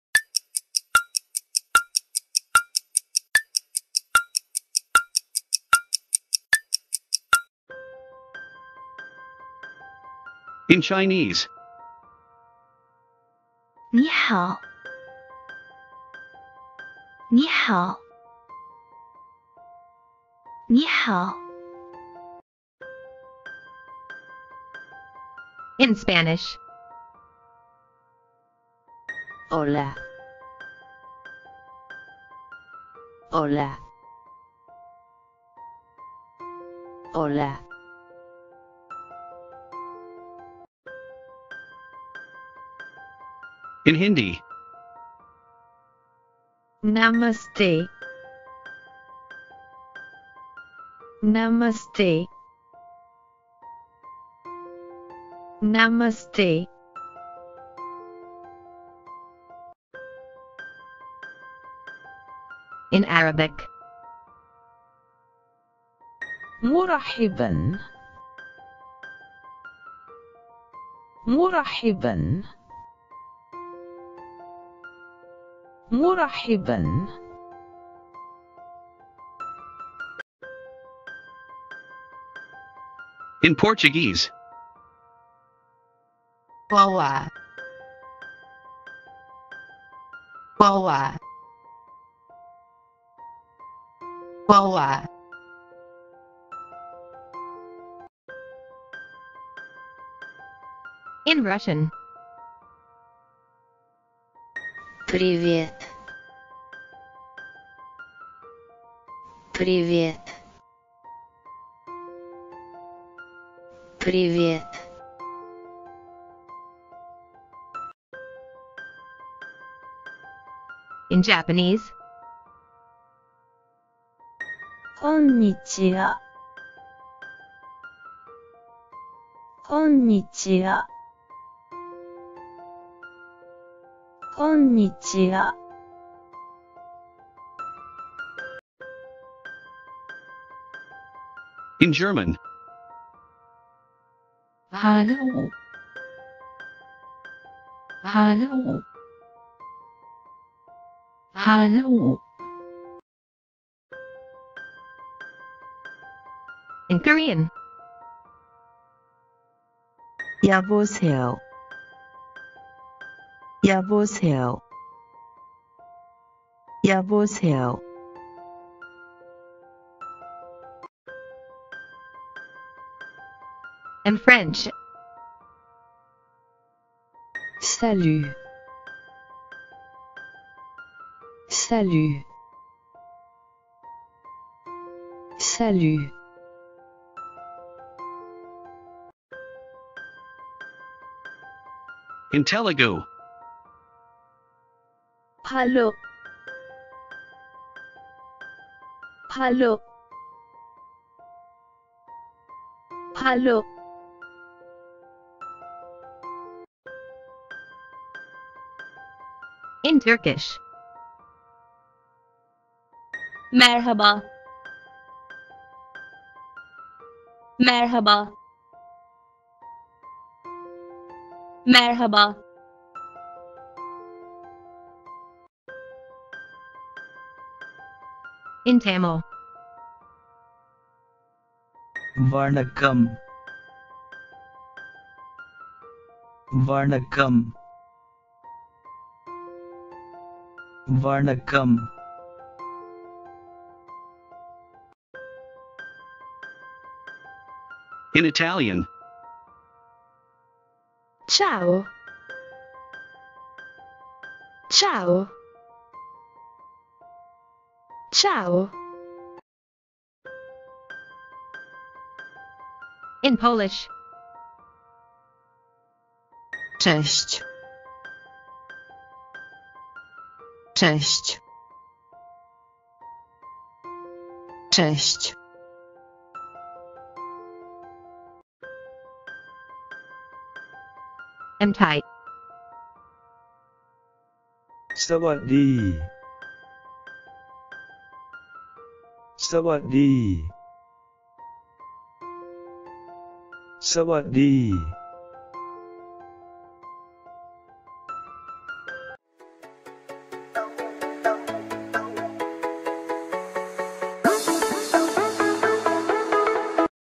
Wish Hello - IN TOP LANGUAGES
(00:07) hello In Chinese is 你好 (00:22) hello In Spanish is Hola (00:40) hello In Hindi is नमस्ते (00:59) hello In Arabic is مرحبا (01:20) hello In Portuguese is Olá (01:38) hello In Russian is Привет (01:57) hello In Japanese is こんにちは (02:18) hello In German is Hallo (02:33) hello In Korean is 여보세요 (02:50) hello In French is salut (03:09) hello In Telugu is హలో (03:26) hello In Turkish is Merhaba (03:45) hello In Tamil is வணக்கம் (04:05) hello In Italian is Ciao (04:20) hello In Polish is cześć (04:38) hello In Thai is สวัสดี